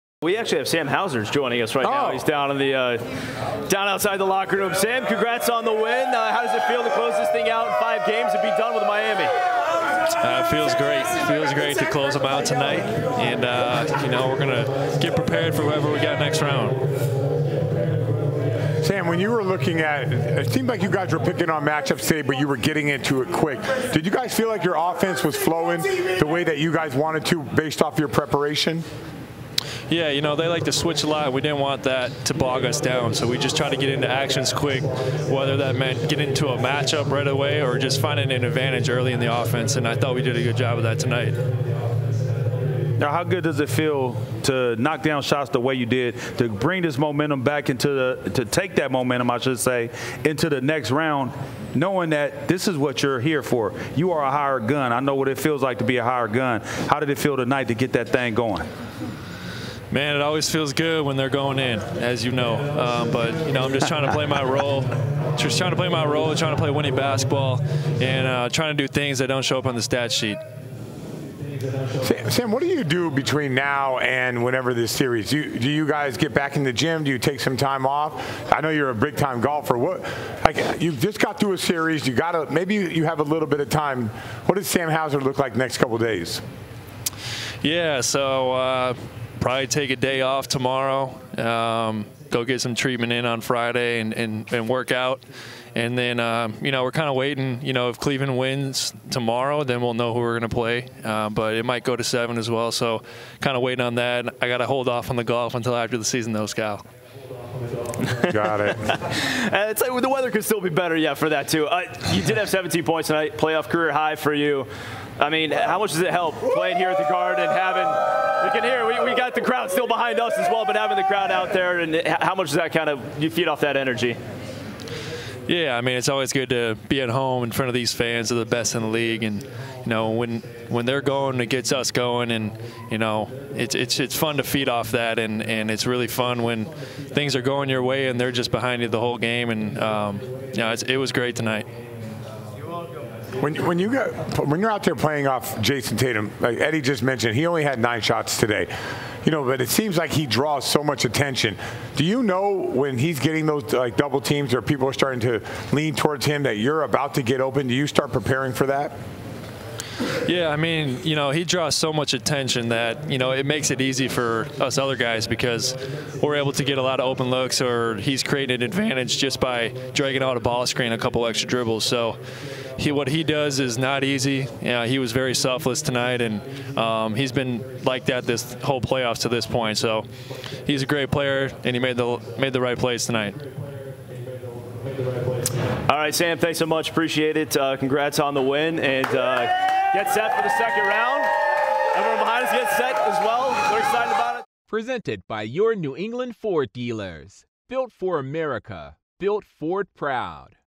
EXCLUSIVE INTERVIEW: Sam Hauser on scoring playoff career-high 17 pts in close-out win vs. Heat
Sam Hauser joins Eddie House, Brian Scalabrine and Tom Giles following the Celtics' series clinching win over the Miami Heat in Game 5. Hauser shares his thoughts on the win, Jayson Tatum's selflessness, and the importance energy of the Boston crowd in the series.